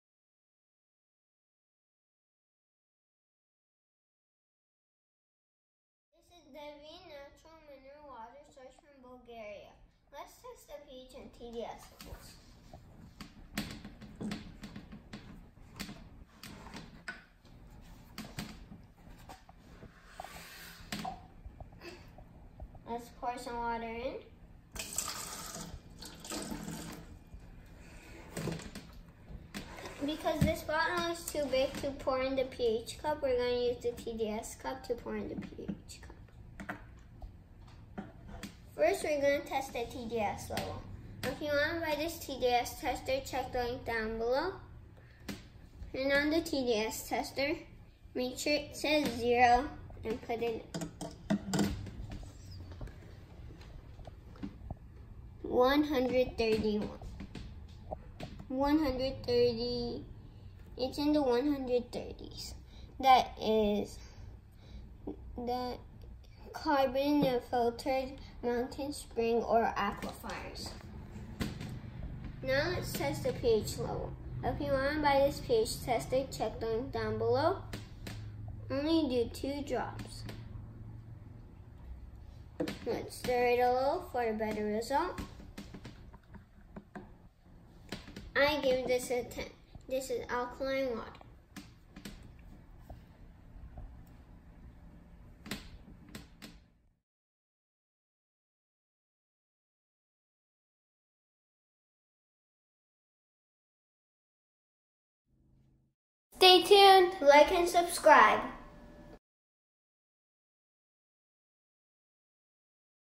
Devin Mineral Water test - pH and TDS (Bulgaria)
Natural Mineral water sourced from Bulgaria

Results:
pH: 10
TDS: 136